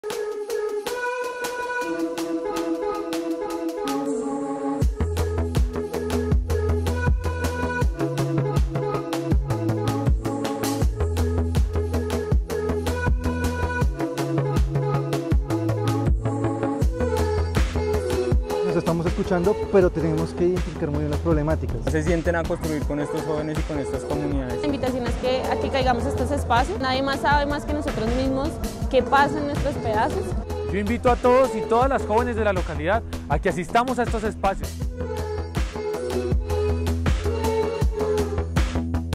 Tuvimos nuestro primer encuentro de GobiernoJoven
En nuestro primer encuentro de GobiernoJoven escuchamos a los jóvenes de Ciudad Bolívar y compartimos con ellos nuestra oferta en educación, trabajo, cultura y deporte.📚🏀

Continuaremos recorriendo las diferentes localidades de la ciudad para acompañar a la juventud bogotana.🙌🏼